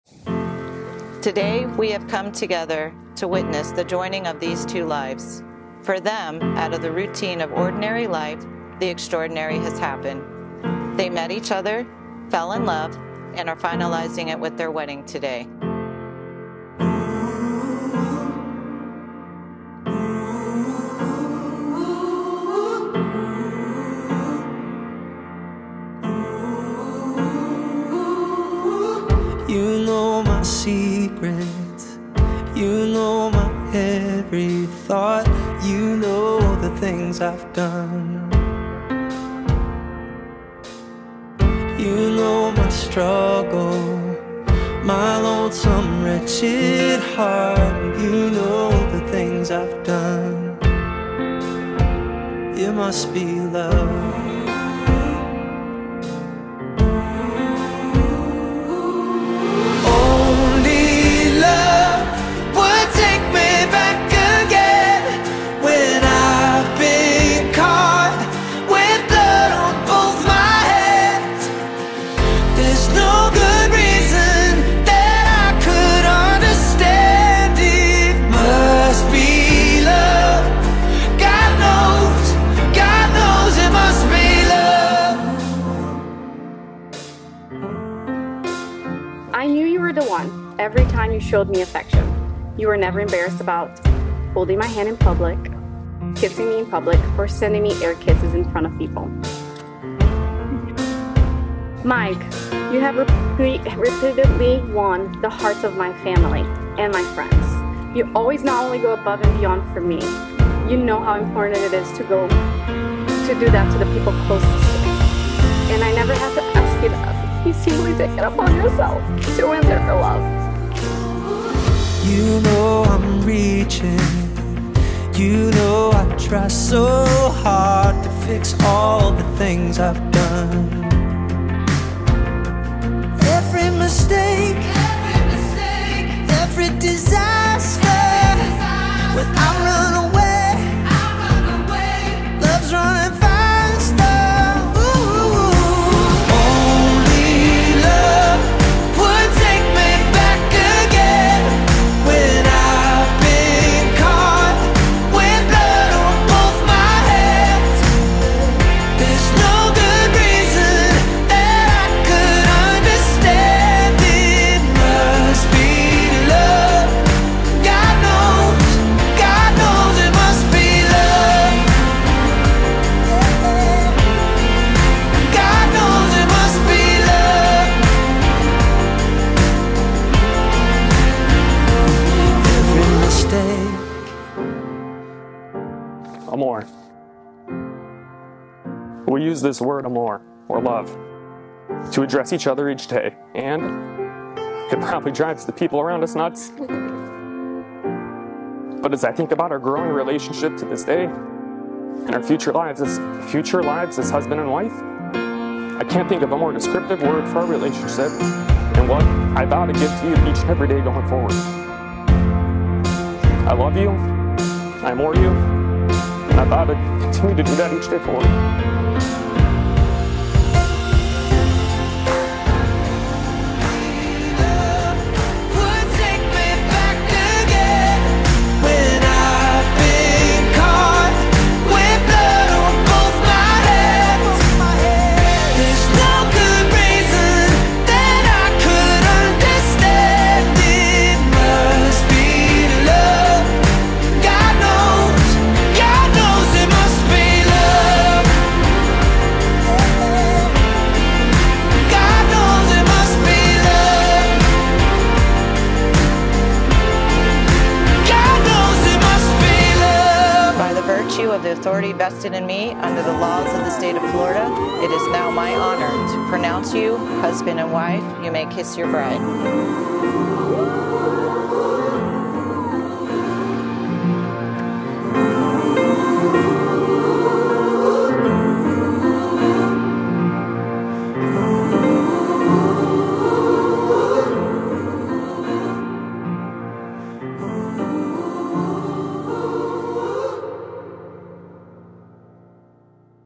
The Dog Played a Big Role in this Beautiful Florida Keys Wedding
This was a beautiful wedding in the Florida Keys, people came from all over and the their dog bear was a big part of their day as well.  This was one of my favorite weddings so far.